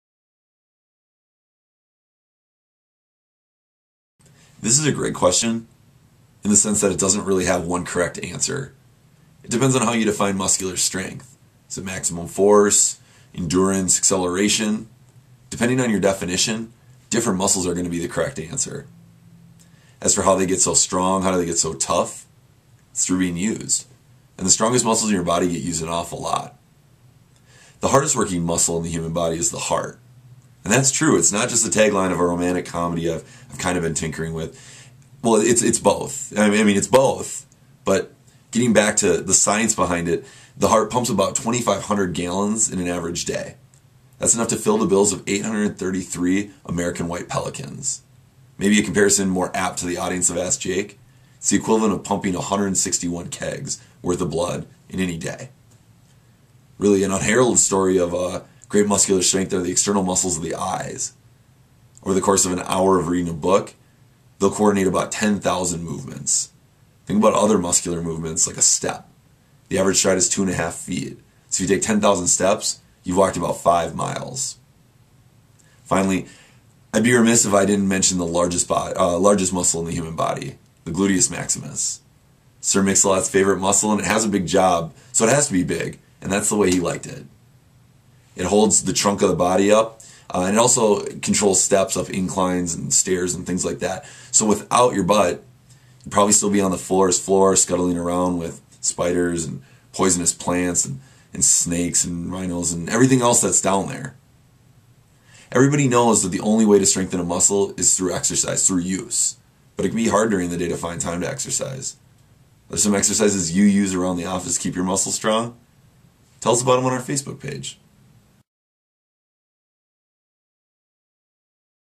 Ask Jake - What's The Strongest Muscle in Your Body?
In the latest video installment of Ask Jake, we discuss taglines for romantic comedies and what might be crawling on the jungle floor while listing the strongest muscles in the human body.